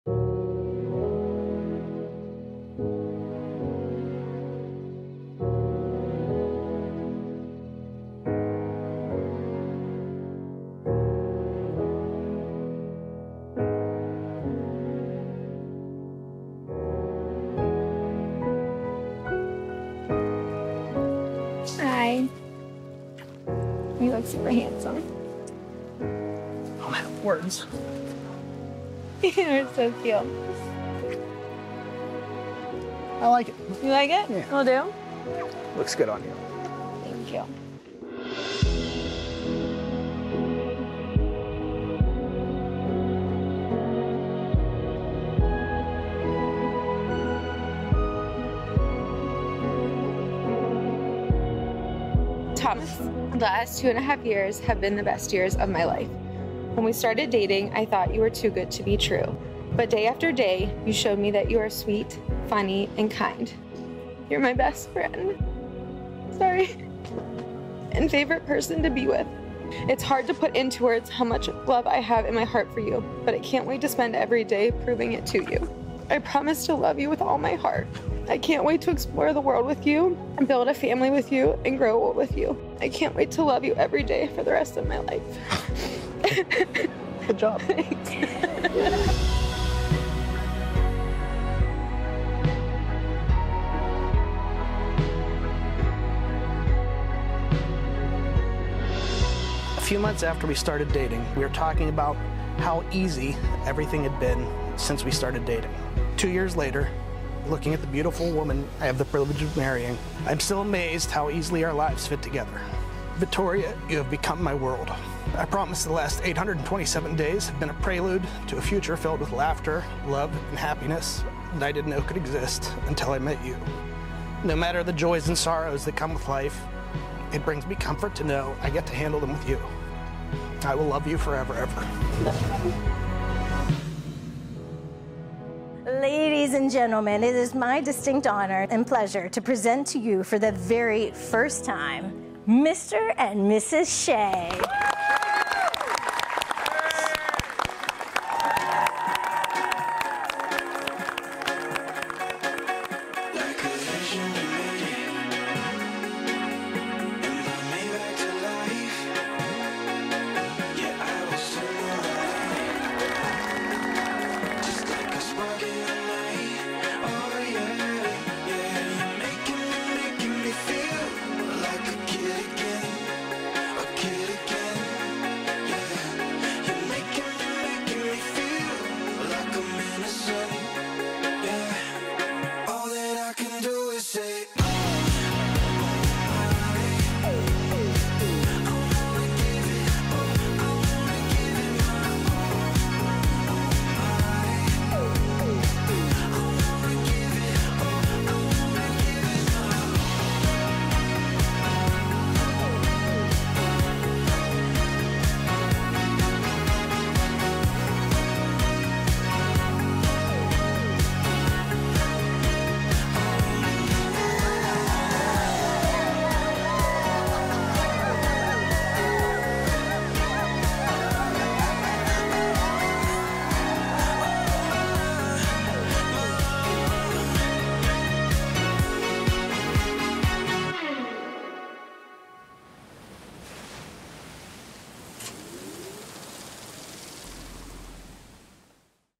Heartwarming Vows at Charleston, SC Wedding
These vows will get you! And that first look - too sweet! Tori & Tom celebrated their love for each other during a beautiful ceremony and reception at the always beautiful Magnolia Plantation and Gardens. Their love story started in the beginning of 2020 and quickly evolved into a strong bond. They spent all of quarantine together and after 6 months of dating Tom sold his house, they moved in together and got engaged the following spring. They wanted a destination wedding that was close enough for guests to travel to and Tori and Tom’s favorite numbers are 6 and 4, so they planned for a Charleston wedding on 6/4. We loved being a part of such a beautiful day filled with love, laughter and happy tears. Capturing all of the small moments, big moments and everything in-between is why we do what we do. We are so incredibly happy for these two lovebirds and can’t wait to see what comes next for them. Cheers to the Sheas!

Venue: Magnolia Plantation and Gardens
Planning/Florals: MOD events
Photography: Sloan Photography
Videography: Palmetto Wedding Films
DJ: Bunn DJ Company Charleston
Hair & Makeup: Silhouette On Site
Catering: Duvall Catering & Events
Bar: Brand Bars and Events
Dessert: The Cake Stand Charleston, SC & Oh So Gelato - Artisanal Gelato & Sorbet - Cart, Catering, & Wholesale
Officiant: Lindsey Katora-Pityk
Lighting: Lowcountry Entertainment
Transportation: Lowcountry Valet
Rentals: Snyder Rentals
Stationary: Hello Beautiful Stationery
Bridal Gown: The Exquisite Bride
Groom Attire: Men's Wearhouse